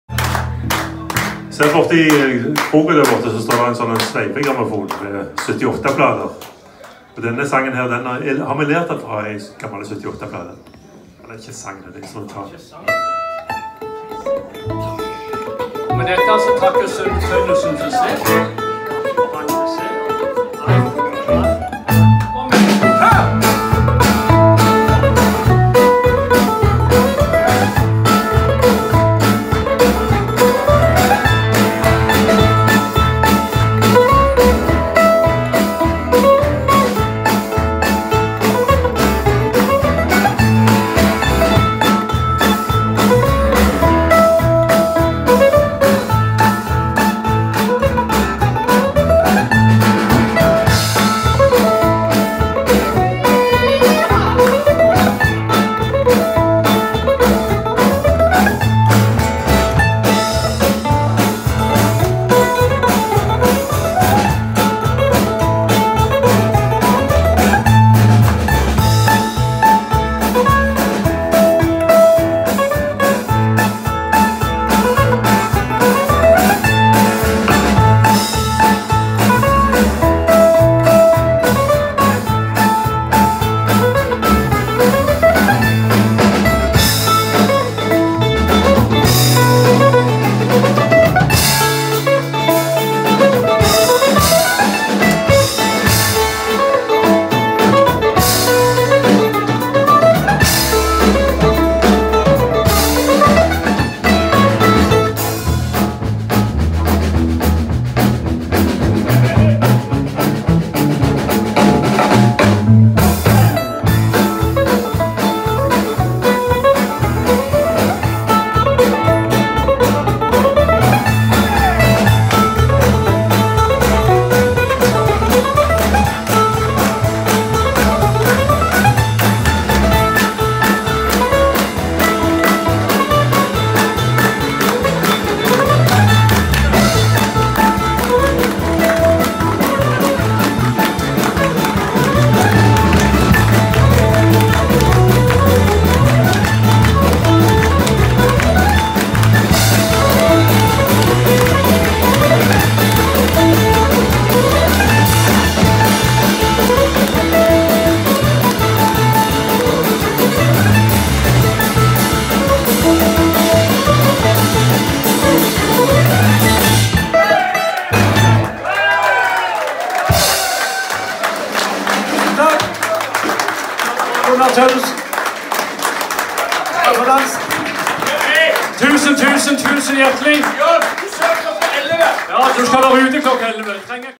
Låvaberget live 19.07.19
Låvaberget holdt konsert i den nedlagte butikken til gitarist John Vigrestad. Stinn brakke og god stemning.